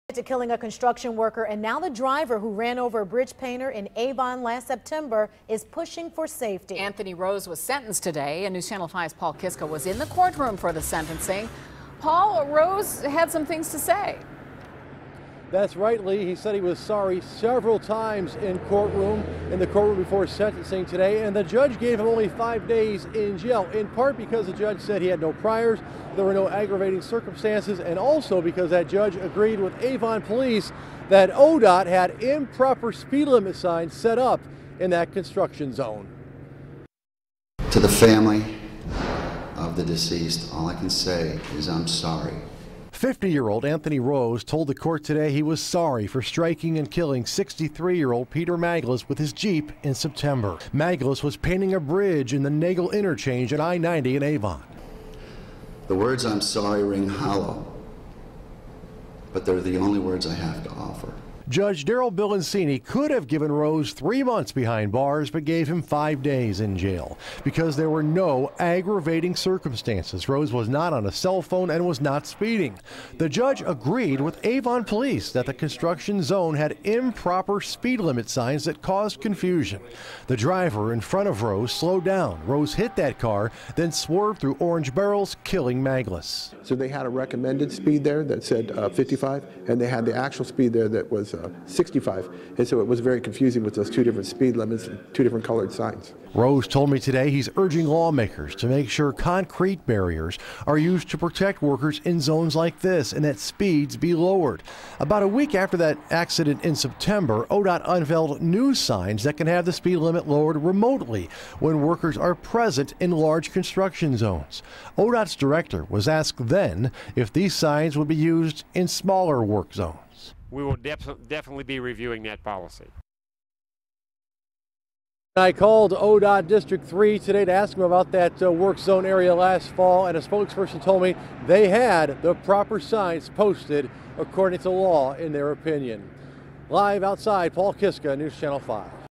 Man gets 5 days in jail for fatal crash
A 50-year-old Brecksville man will spend five days in jail for a September accident that killed a bridge painter.